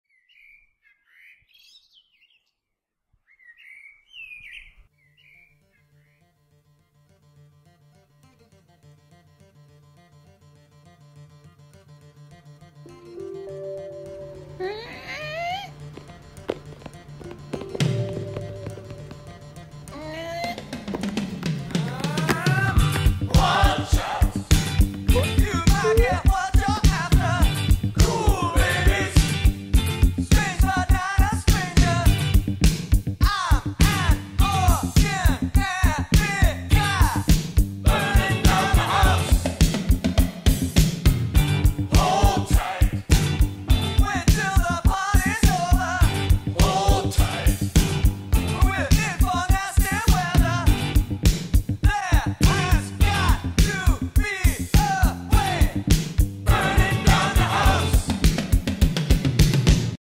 Heart in a Trunk- A Stop Motion Animation By Jennifer Strings
What is that scuttling across my floor? It’s Skittles, the anxious heart. Don’t worry buddy, I’m with ya...

Jennifer Strings= known.asmyself@Instagram